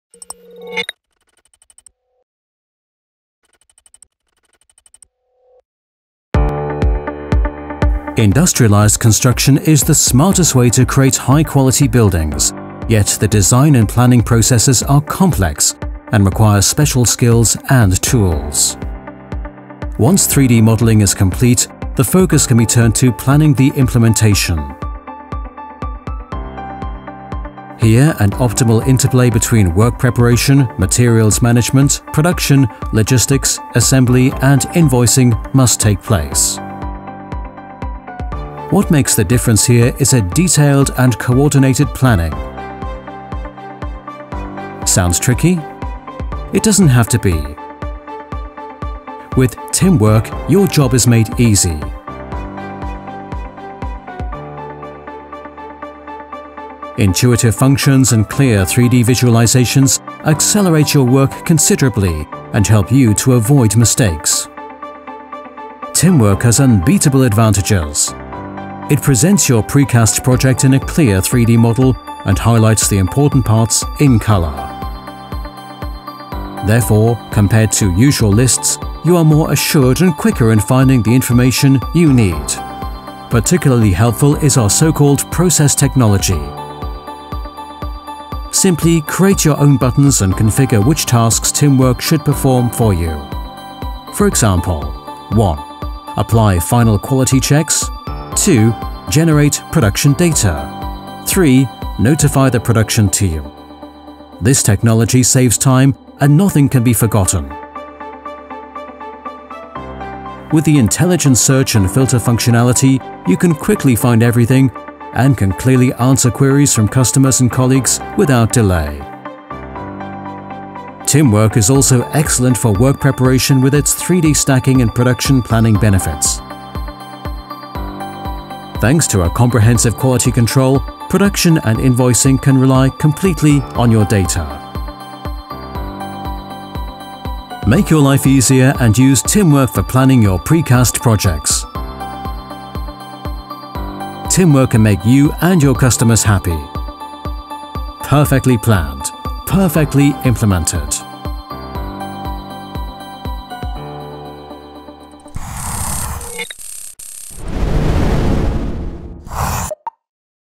Tim - ALLPLAN's work preparation solution for precast processes
Tim is an excellent tool for work preparation with its 3D stacking and production planning benefits. Thanks to comprehensive quality control, production and invoicing can rely completely on your data. Particularly helpful is our so-called process technology: simply create your own buttons and configure which tasks Tim should perform for you. This technology saves time and nothing can be forgotten.

Your benefits with Tim
🔸apply final quality checks
🔸generate production data
🔸notify the production team.